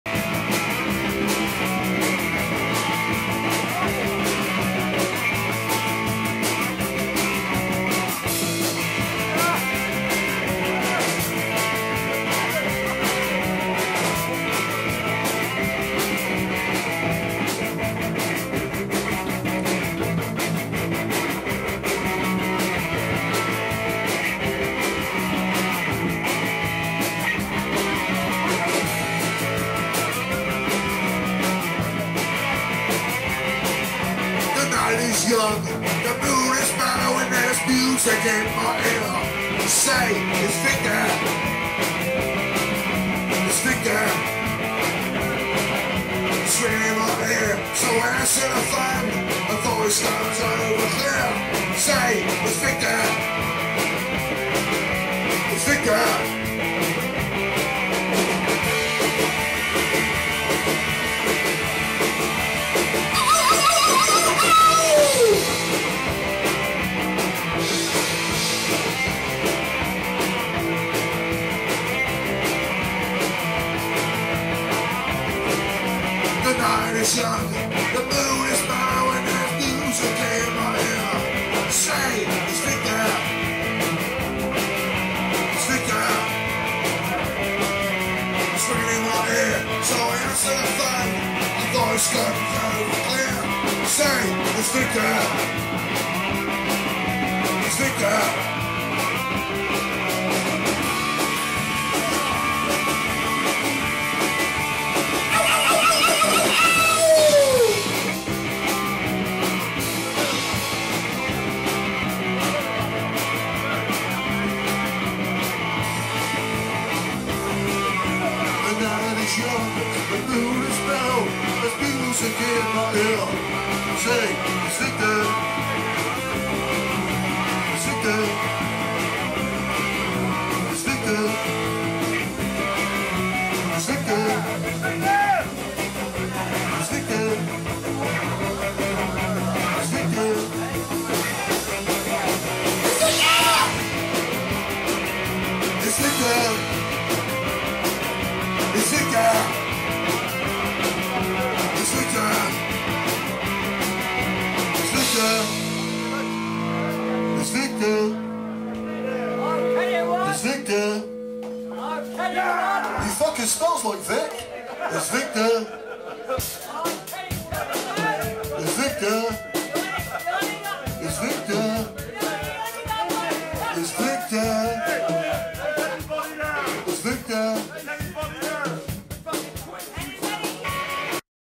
johnny bodman at the trout
johnny bodman heckling with the sicknotes, hes been trying to get through to vic for the last 30 years